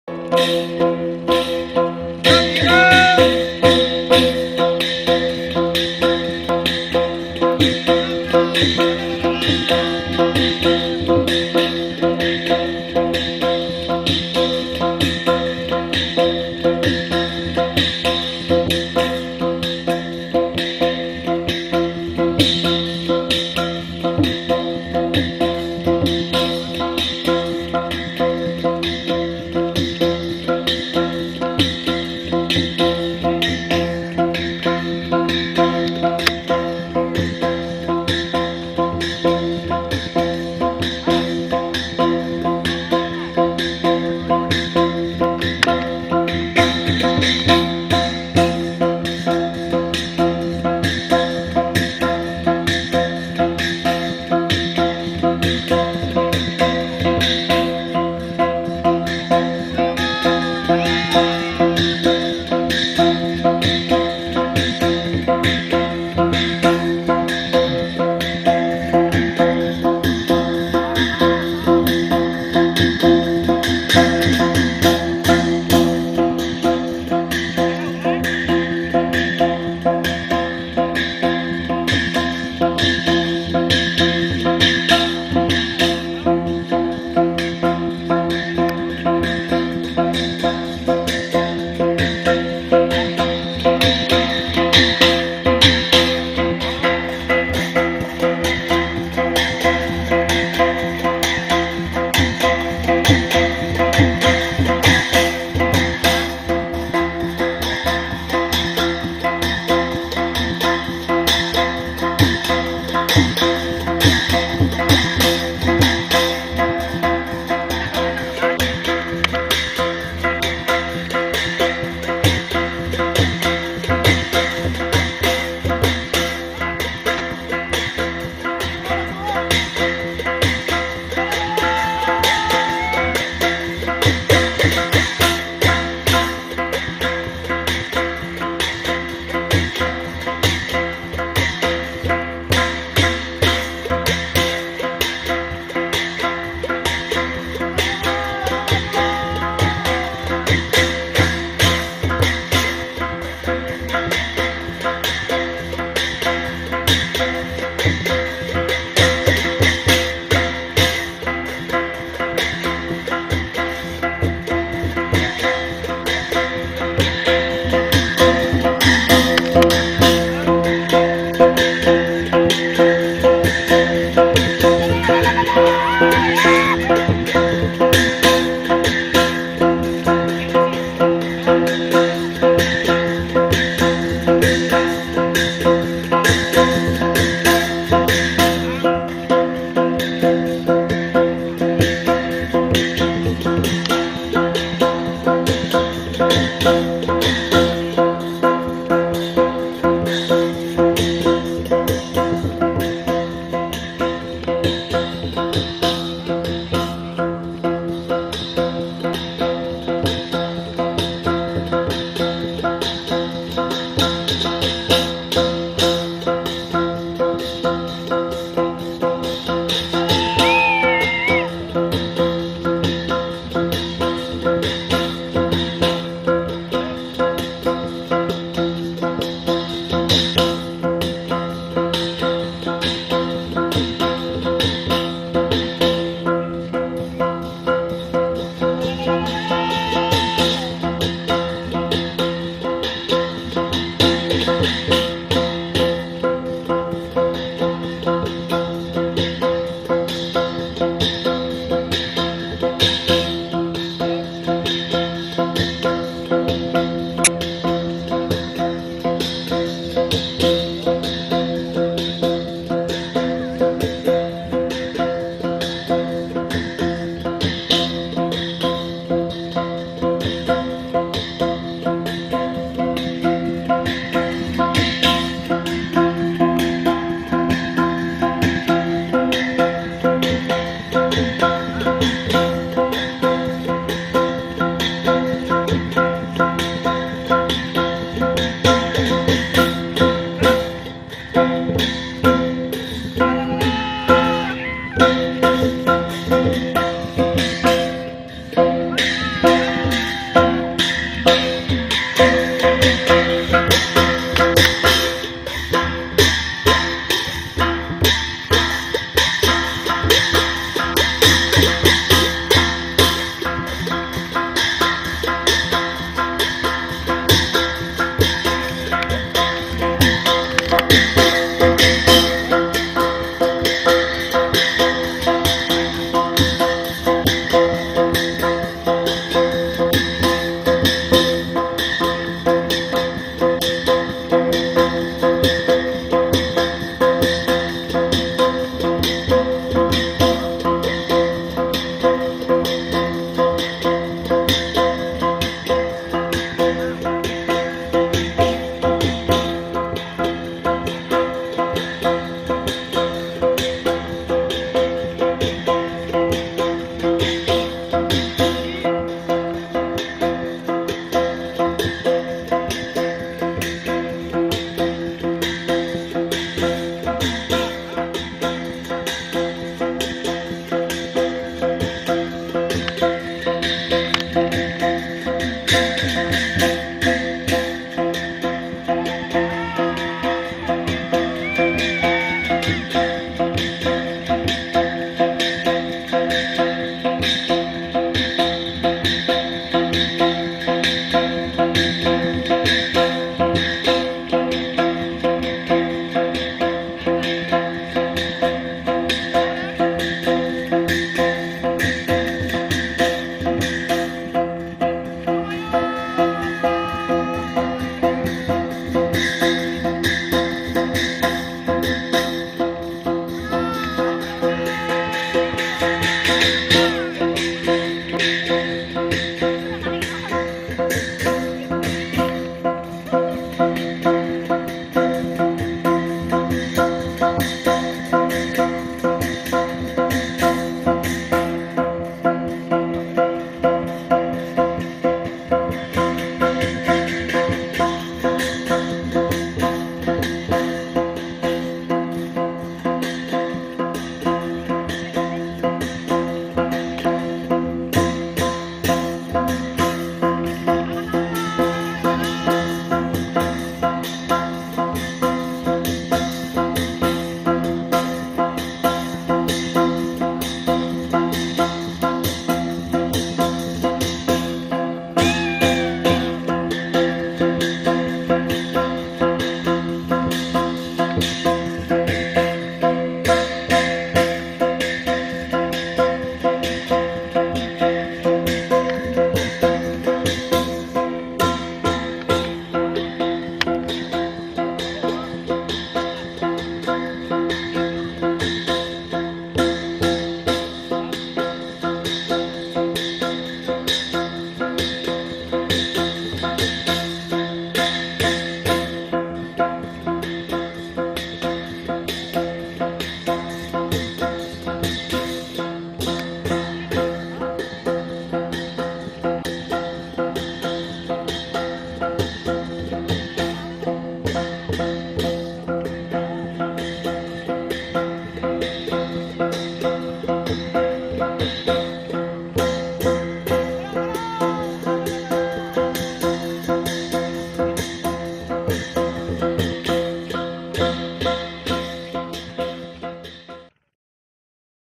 Dai dance: "Daang Ga - Mao Sao" as one of common "Daang Ga Dai"
Dai dance: "Daang Ga - Mao Sao" with "Gon Mon Sung" as one of common "Daang Ga Dai".
For clear image to resolution 480p at setting.